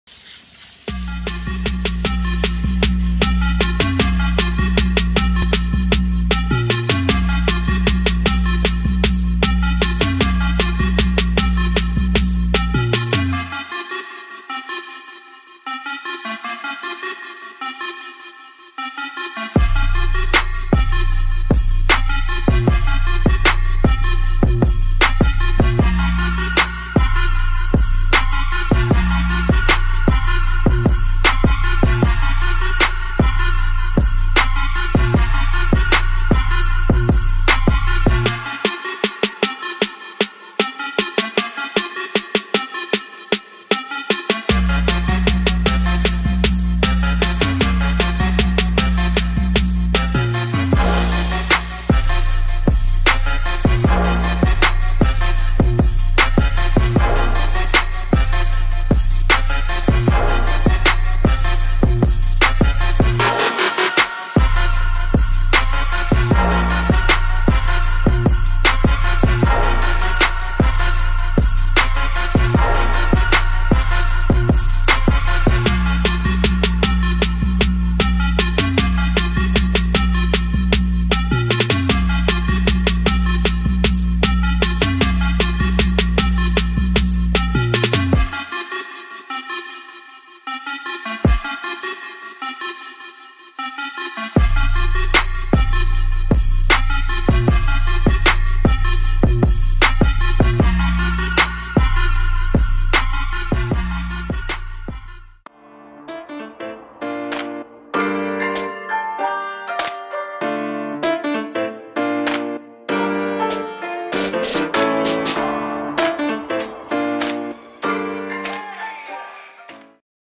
Transforming a Military UAZ 452 into a Beach Camper - Paint Job Transformation | ABH Customs 🇻🇳
Transformation of an old military truck into a cool beach bus in a VW Camper Style for decorative purposes at Coco Beachcamp.

December 2016 - Lagi, Vietnam 🇻🇳

Keep up to date with all my projects...